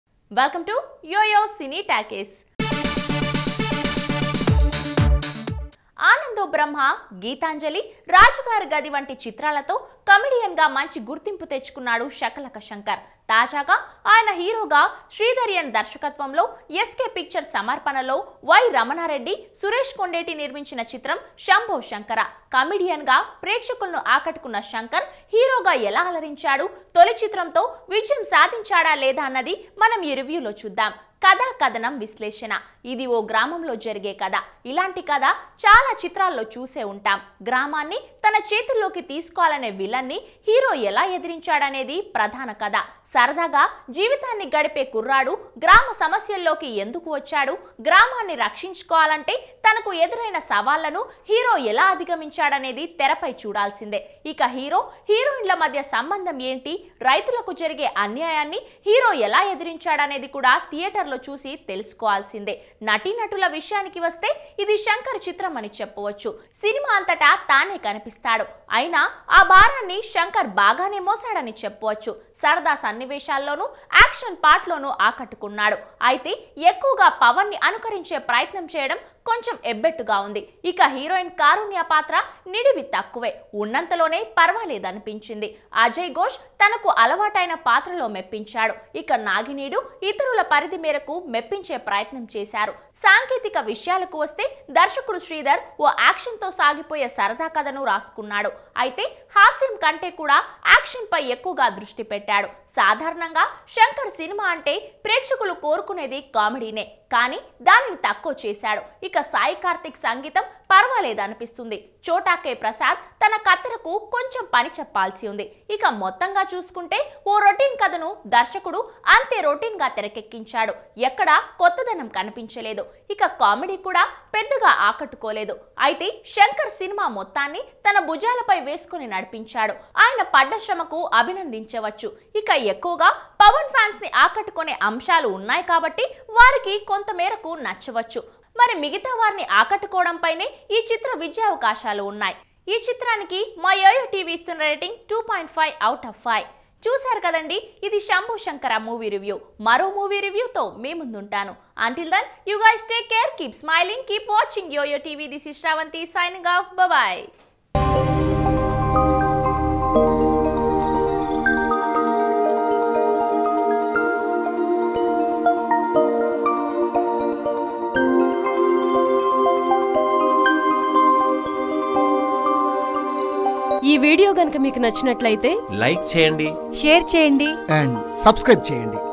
Shakalaka Shankar's Shambo Shankara Review | Karunya | YOYO Cine Talkies
shambo shankara review,

Shambho Shankara is a Telugu movie starring Shakalaka Shankar and Karunya Chowdary in a prominent role. It is a drama directed by N. Sreedhar.

Surabhi.